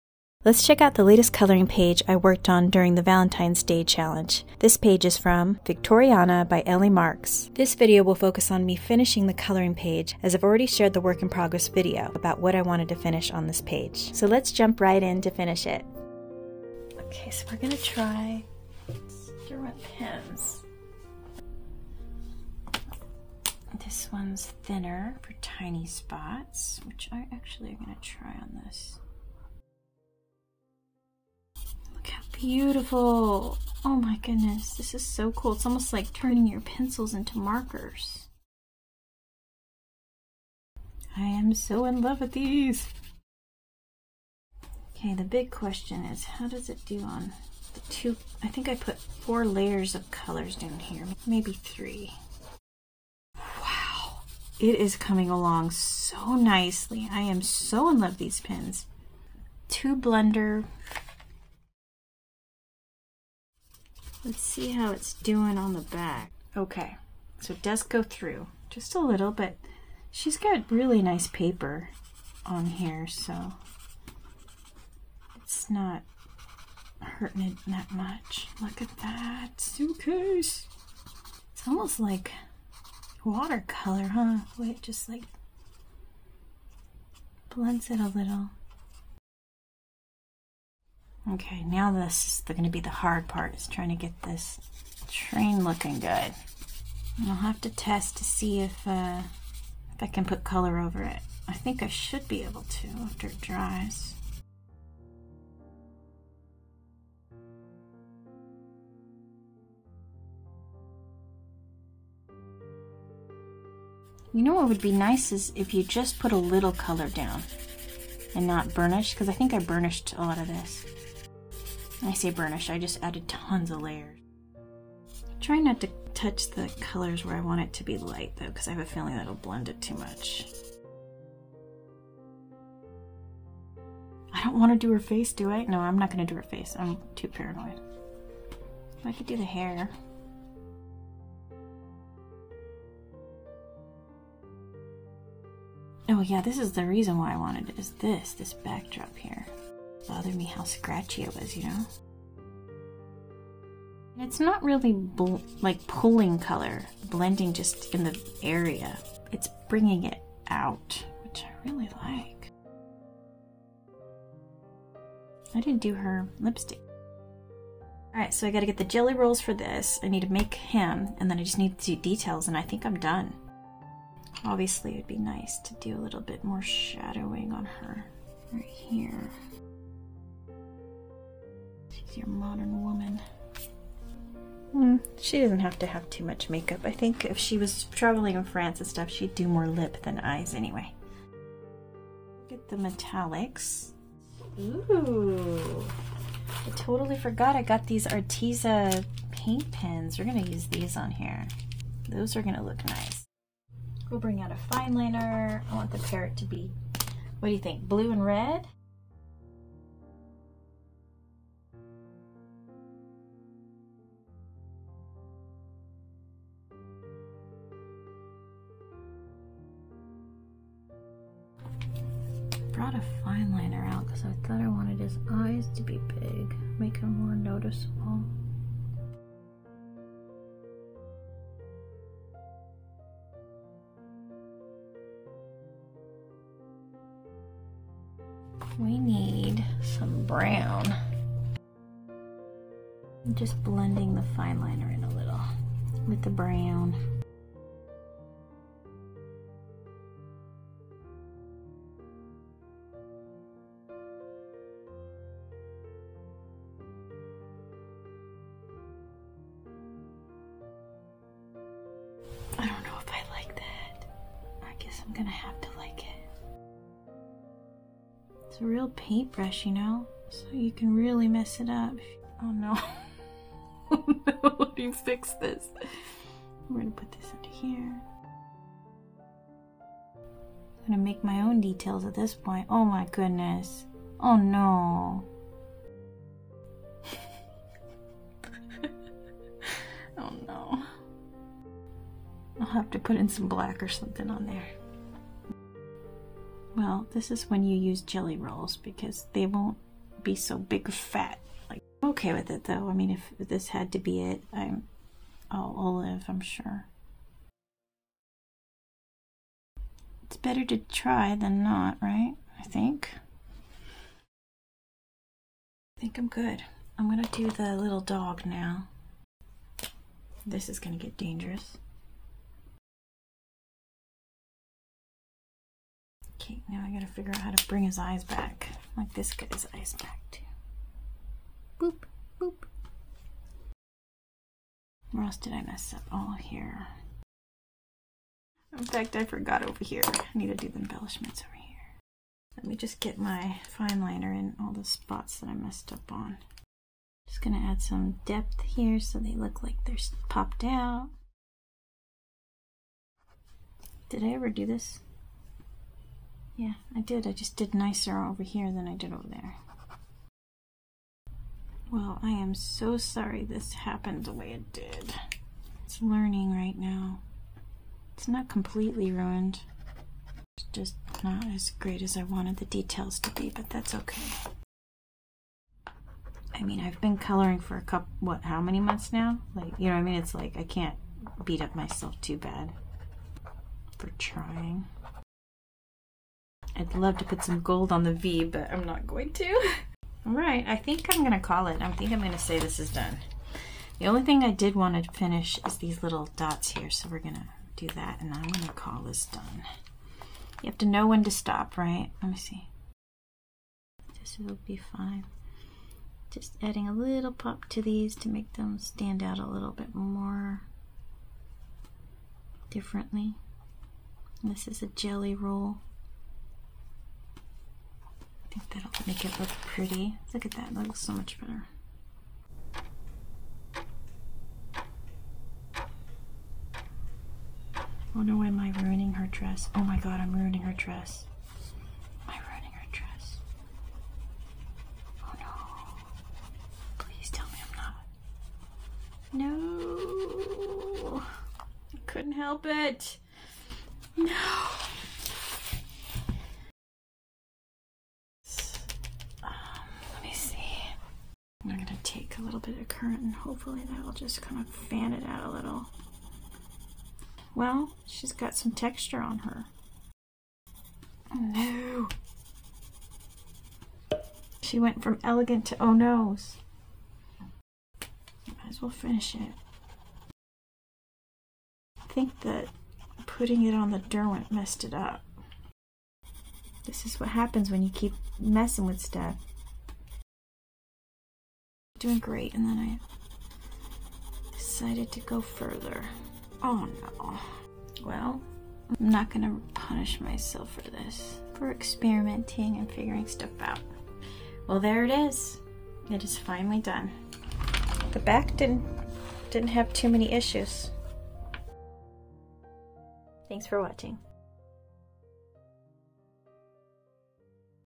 Coloring the Past: Victorian Train Scene
Embark on a coloring journey with me as I bring to life a train scene from the Victoriana coloring book by Ellie Marks. In this video, I try a couple of new coloring supplies, such as Derwent blending pen and Arteza paint brush metallic pens. Despite a few unexpected challenges, this page still came out lovely.  I am so grateful for a coloring page that offers these types of Victorian Lifestyles! I hope you will join me on this coloring journey, and perhaps learn something from my own mistakes. Side note: I did color in the parrot's cage too, but the video cut out!

If you are just joining me, this is a page selected as part of a series of videos dedicated to my Valentine's Day challenge where I attempt to complete 7 coloring pages in 2 weeks. Here's the video series in case you missed them.

Valentine's Day - Work In Progress Pages where I share a checklist of what is needed for me to consider the pages completed.

Materials used:
- Evazar London coloring pencils
- Derwent Blender Pens
- Arteza metallic paint brush pens
- Gelly Rolls
- Posca Paint pen